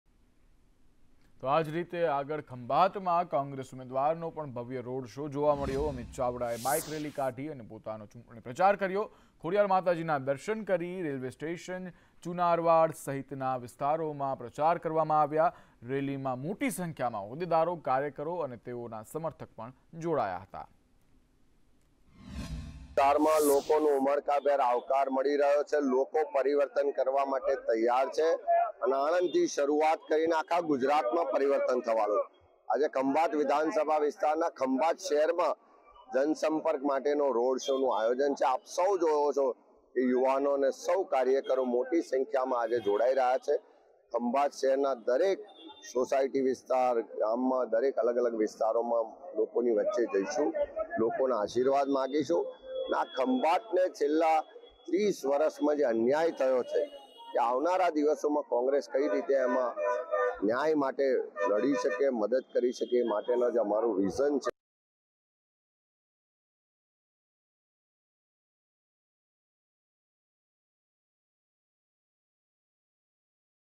Khambhat | ખંભાતમાં કોંગ્રેસ ઉમેદવારનો ભવ્ય રોડ શો યોજાયો | Congress
ખંભાતમાં કોંગ્રેસ ઉમેદવારનો ભવ્ય રોડ શો યોજાયો
અમિત ચાવડાએ બાઈકરેલી કાઢી કર્યો ચૂંટણી પ્રચાર
ખોડિયાર માતાજીના દર્શન કરી બાઈક રેલી કાઢી
રેલવે સ્ટેશન, ચુનારવાડ સહિતના વિસ્તારોમાં પ્રચાર
રેલીમાં મોટીસંખ્યામાં હોદ્દેદારો-કાર્યકરો, સમર્થકો જોડાયા